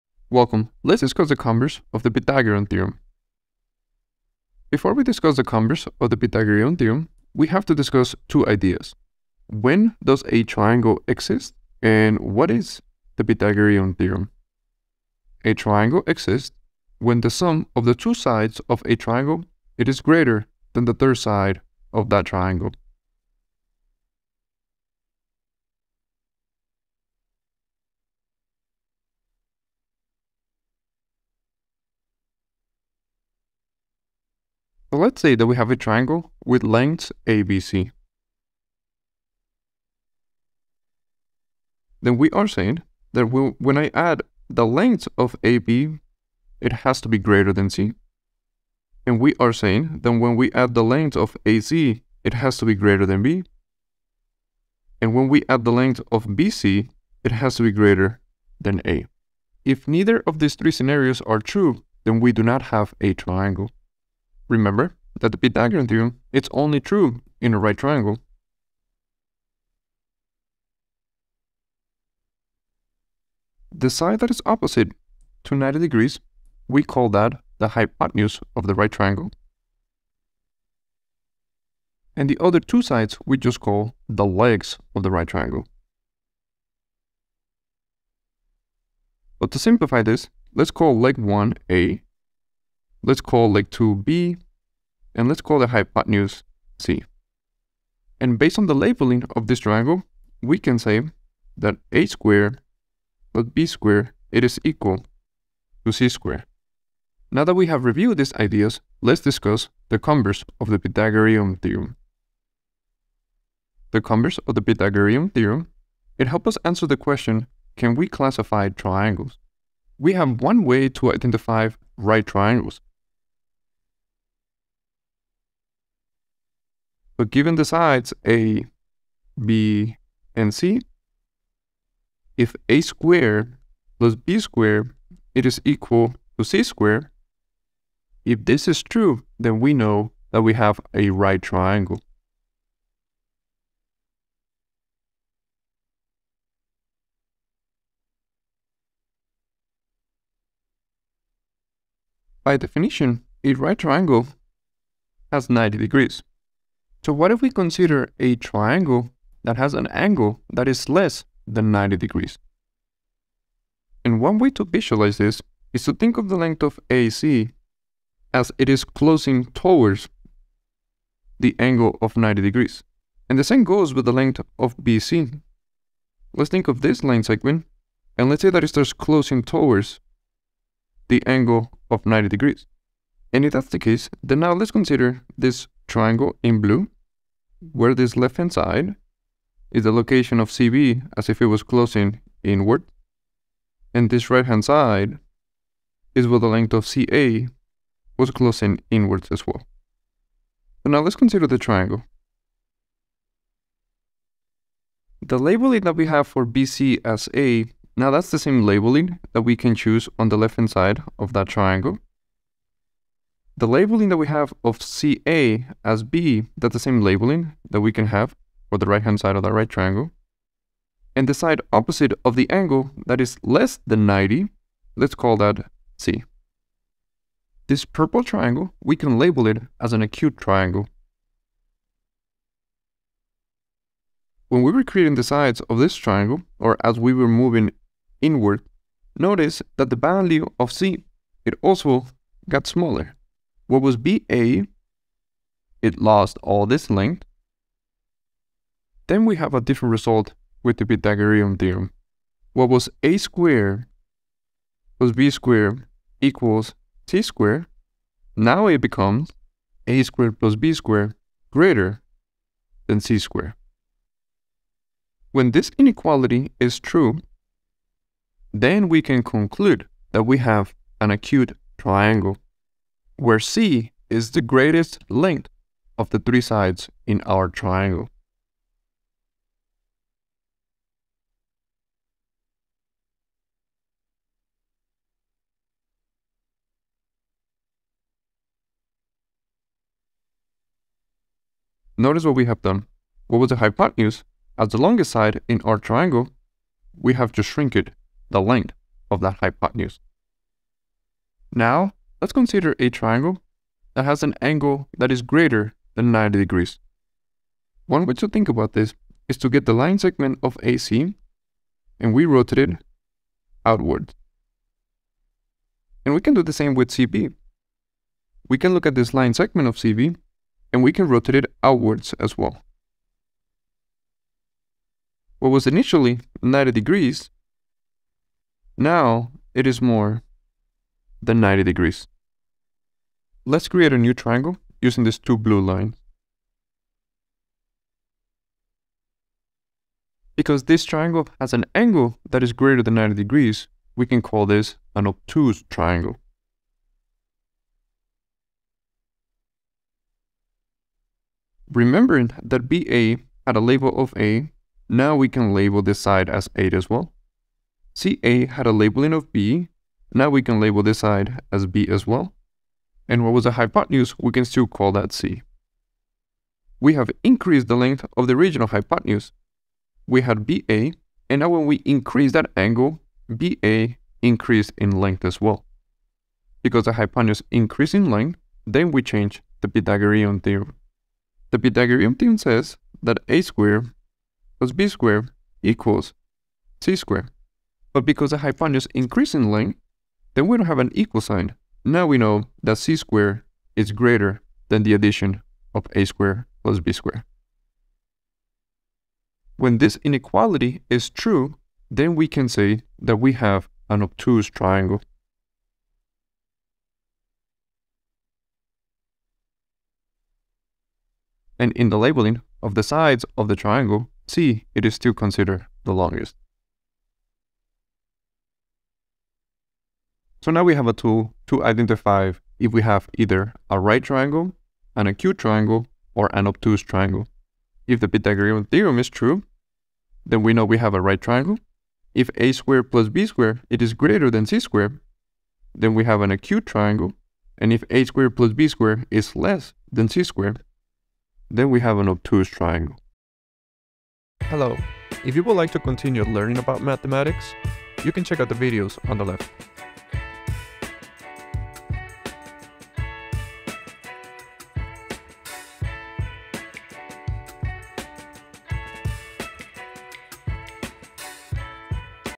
Converse of the Pythagorean theorem (Why is it Usefull?)
Converse of the Pythagorean theorem is a lesson that will  show you how we can classify triangles based on the Pythagorean theorem. The Pythagorean theorem fits in a right triangle, but what about other cases. We will use the converse of the Pythagorean theorem to cover those other cases.

⏰Timestamps⏰
0:00 Introduction
0:05 when do we have a triangle?
1:03 what is the Pythagorean Theorem?
2:00 Identifying right triangles
2:30 Identifying Acute Triangles
5:12 Identifying Obtuse Triangles